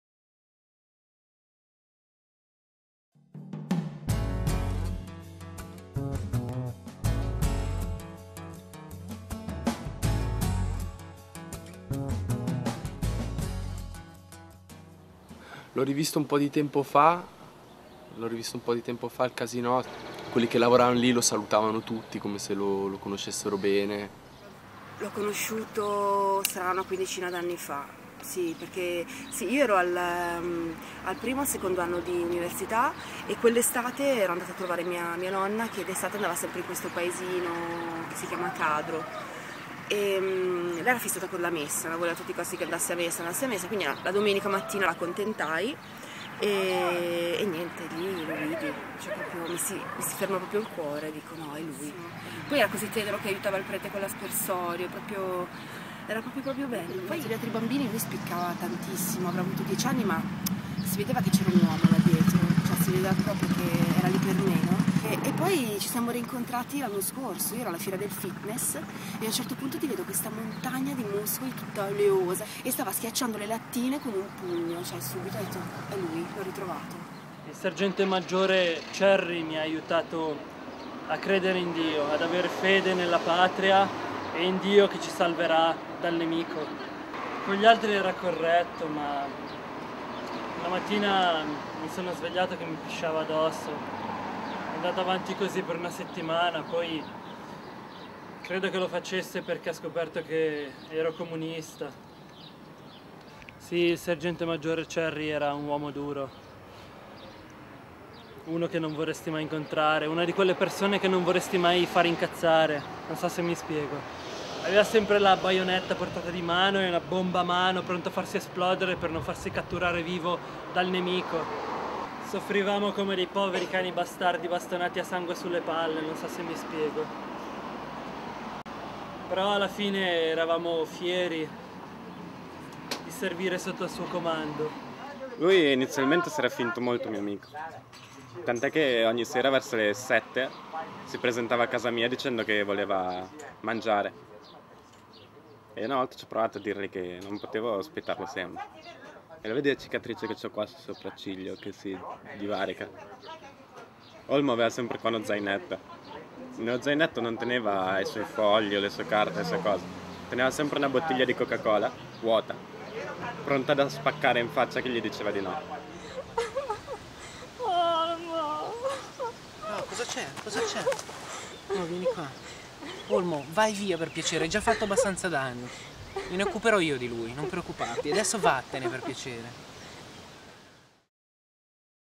Histoires d'O(lm) - capitolo 3
Mockumentary realizzato per il mio 27° compleanno da Simo, Ushi, Ste ed una serie lunghissima di più o meno consapevoli comparse. Grazie! :-)